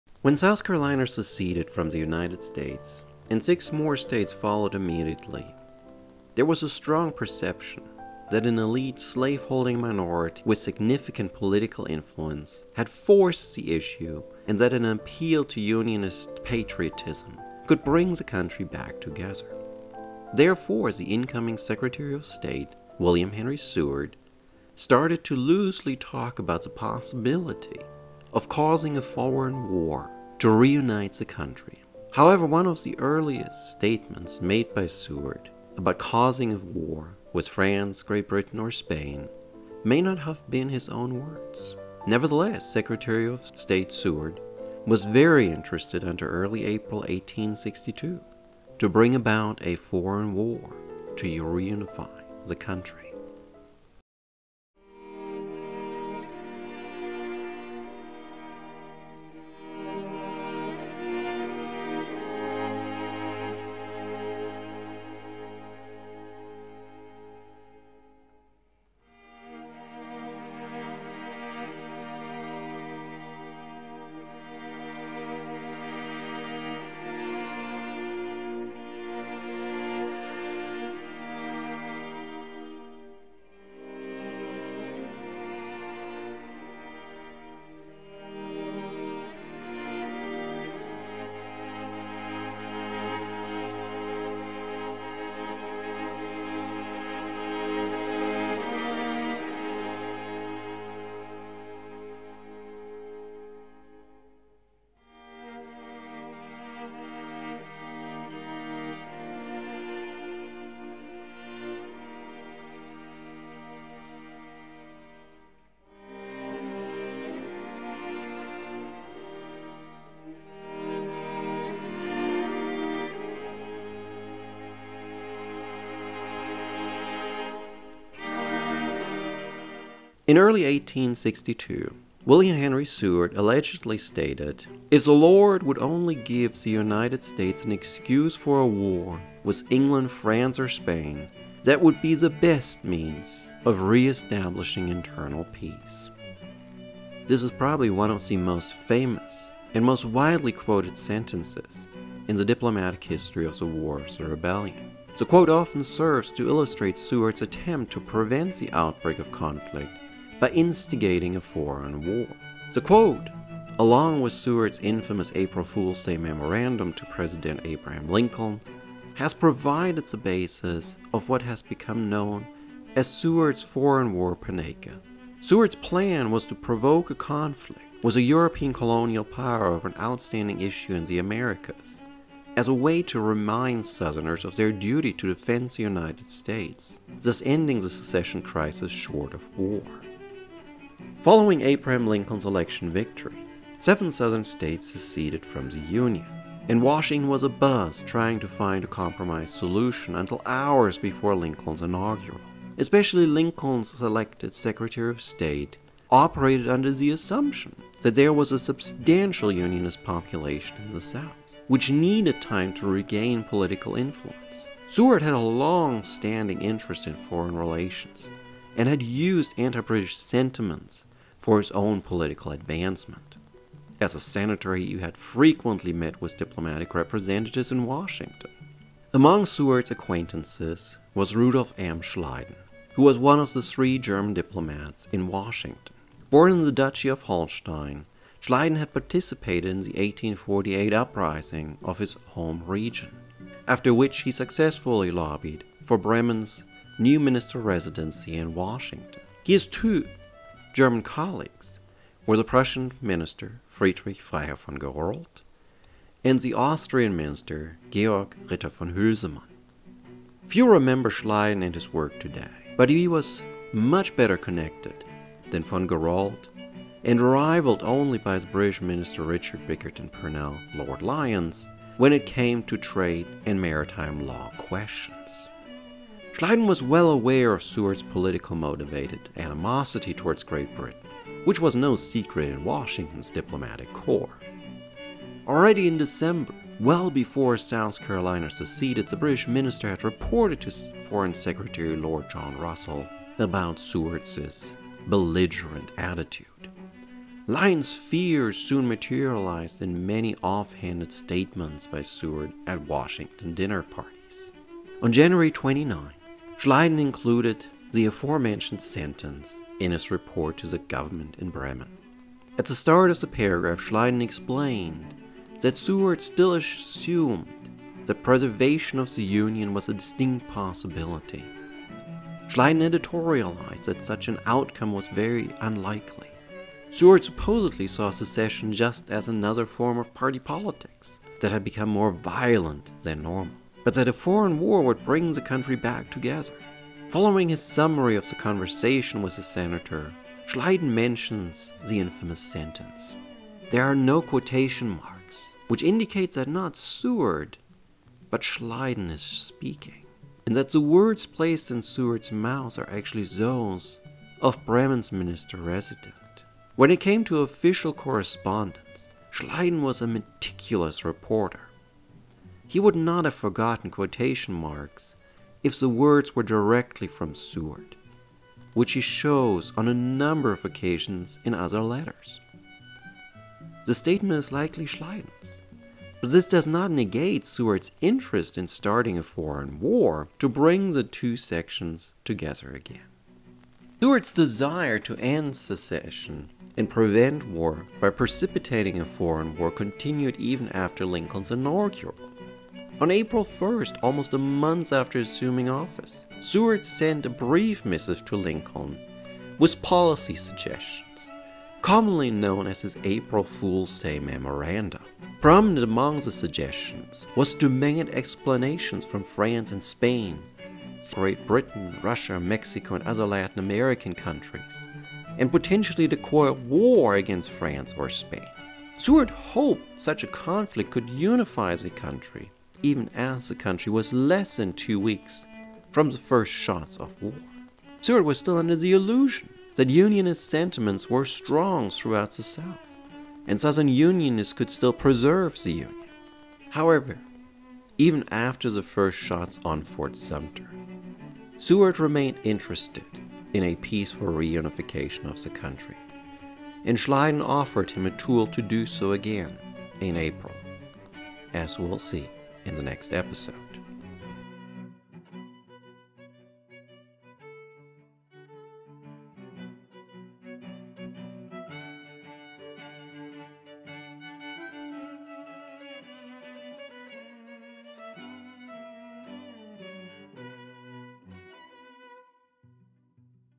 Episode 40--Seward's Foreign War Panacea
In early 1861, the future Secretary of State William H. Seward talk about causing a foreign war with Great Britain, France, or Spain in order to reunite the country. One of the often quoted statements, may not be his as we will explore in the video. Nevertheless, until early April 1861, Seward continued to belief that a foreign war might offer the best means to reunite the United States.